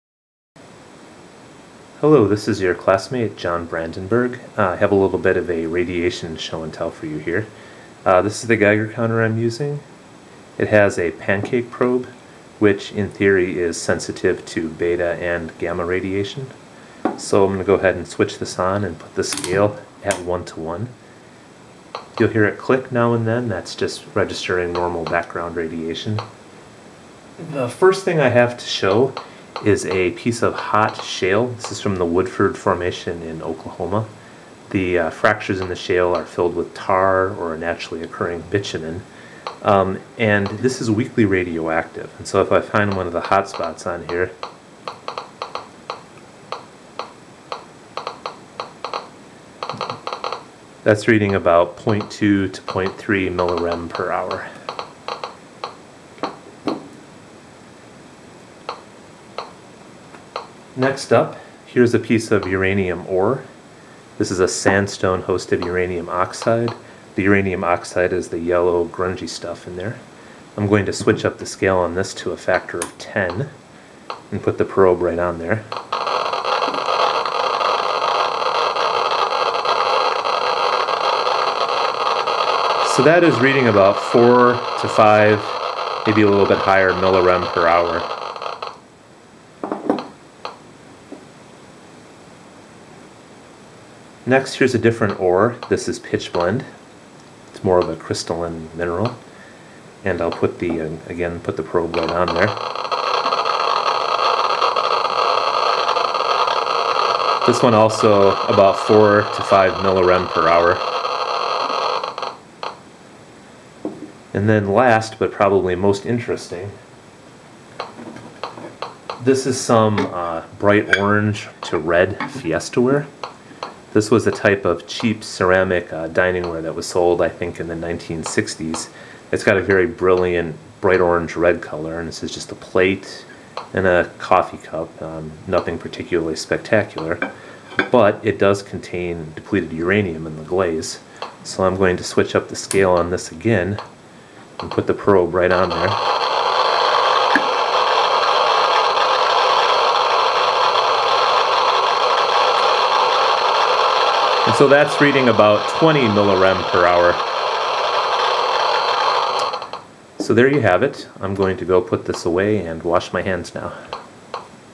Some radioactive rocks and materials
Some radiation show and tell.  The first three examples are naturally occurring rocks that you could go pick up off the ground in Oklahoma or Utah.  The last is some "radioactive red" Fiesta Ware...the name is most definitely literal.